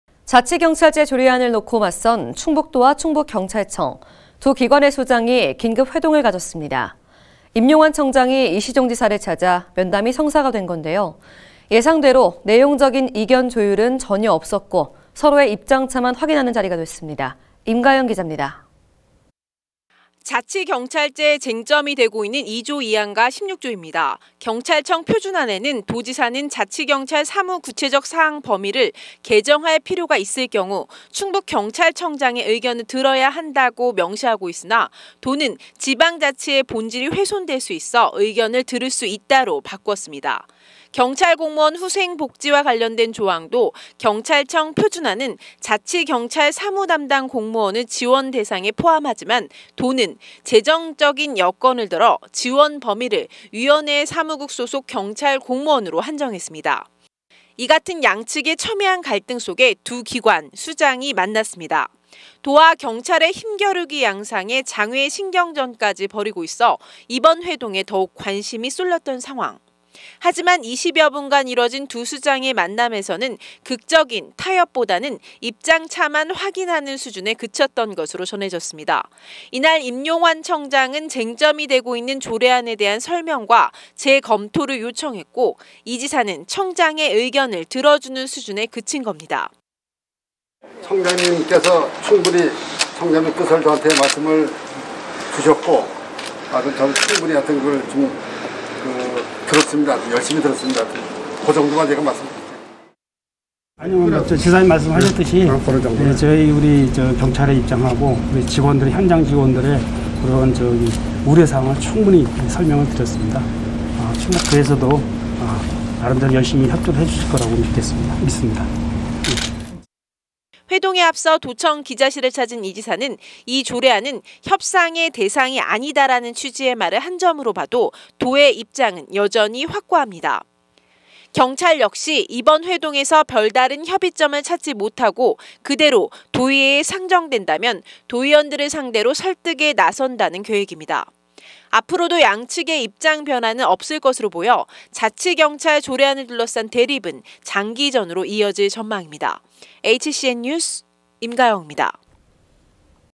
이시종-임용환 두 수장 만났지만...간극은 여전/충북 현대HCN
자치경찰제 조례안을 놓고 맞선
충북도와 충북경찰청,

두 기관의 수장이 긴급 회동을 가졌습니다.

임용환 청장이 이시종 지사를 찾아
면담이 성사가 된 건데요.

예상대로 내용적인 이견 조율을 전혀 없었고
서로의 입장 차만 확인하는 자리가 됐습니다.

임가영 기잡니다.